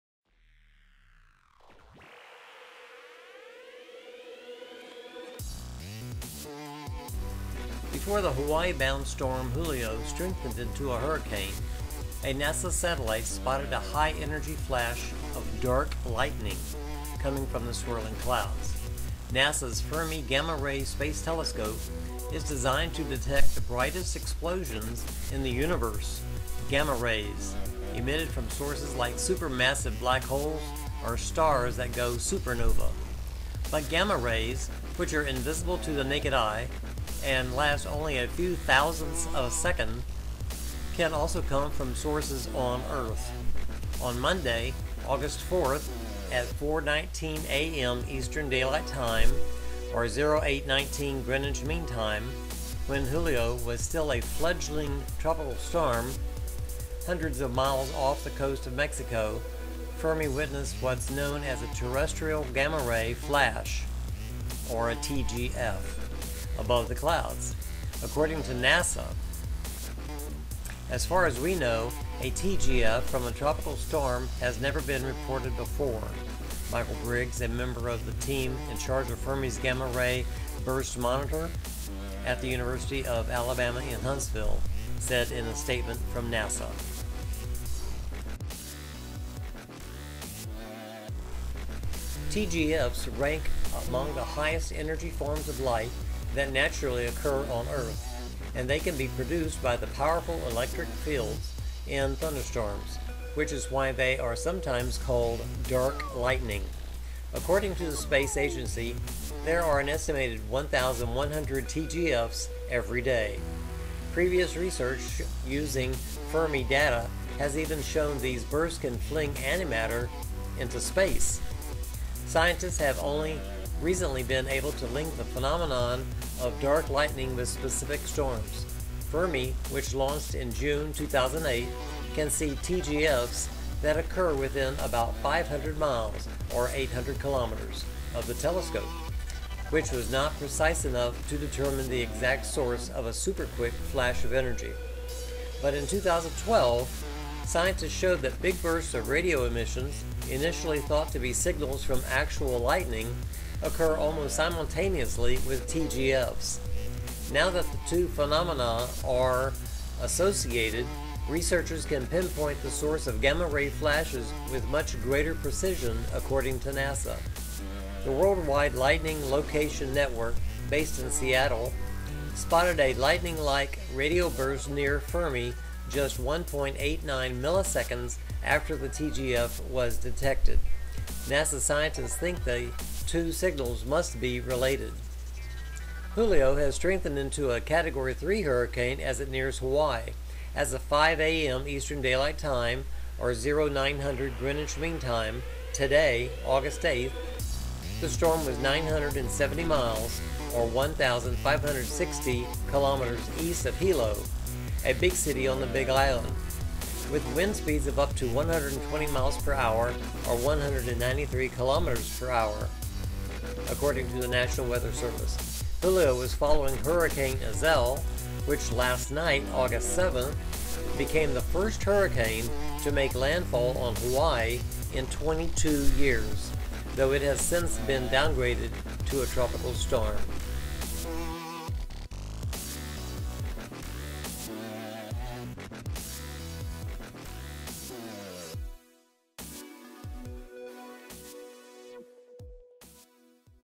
Julio's 'Dark Lightning' Over Hawaii
NASA's Aqua satellite imaged twin hurricanes Iselle and Julio approaching Hawaii.  Before the Hawaii-bound storm Julio strengthened into a hurricane, a NASA satellite spotted a high-energy flash of "dark lightning" coming from the swirling clouds.